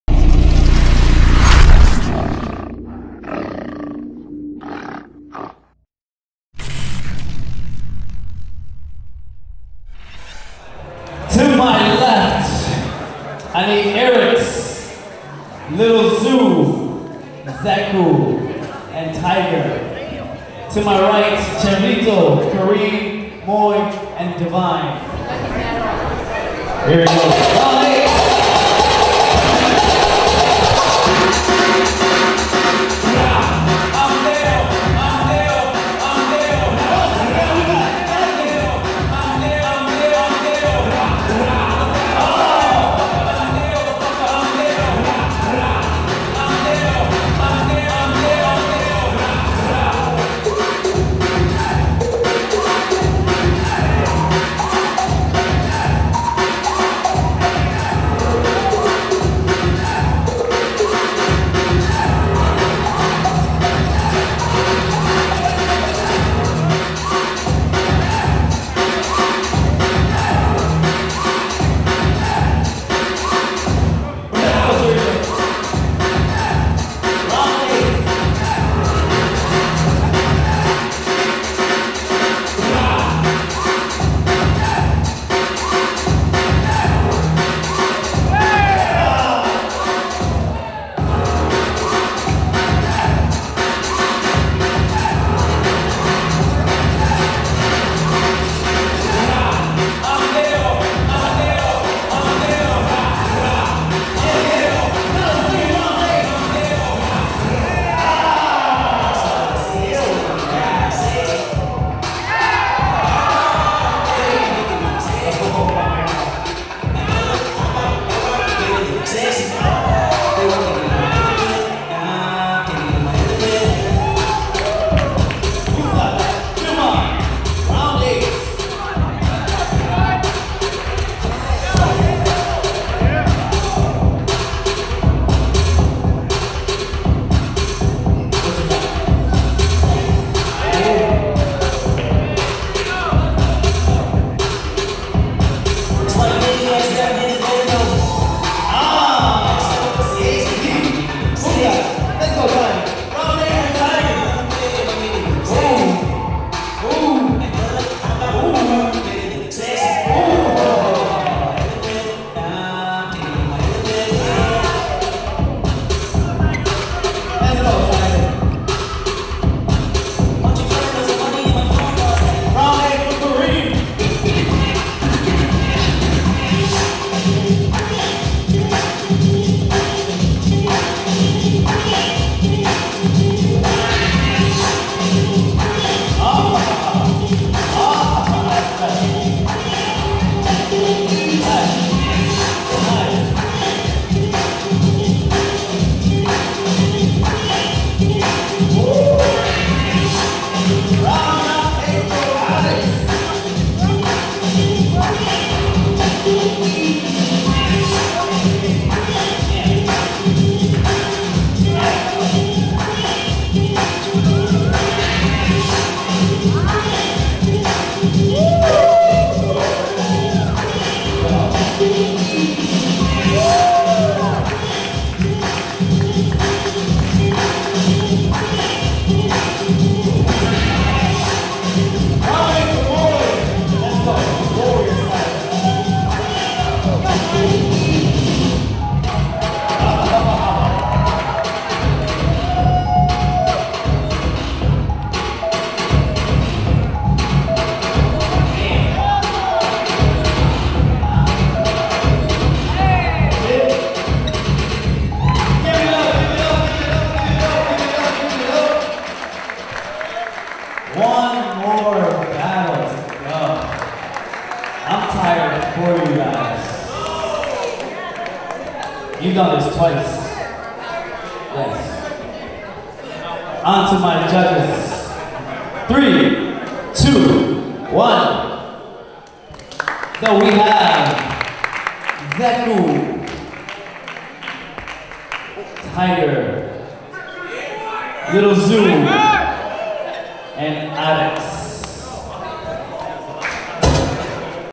Round 8 | Crazy 8s | Sweet Lu's Crazy 8s | Pro Breaking Tour | BNC
Sweet Lu's Crazy 8s

Crazy 8s | Round 8  | 4 v 4

Kareem/Moy/Cheerito/Divine Vs Zeku/Tiger/Lil Zoo/Arex

Winners
Zeku/Lil Zoo/Divine/Kareem

Judges
Jiggz (Supreme Beingz)
Trinity (United Outkast)
Gravity (Team Monster)

DJ
EX (Dynamic Rockers)

Emcee
Sweet Lu (United Outkast / Dynamic Rockers)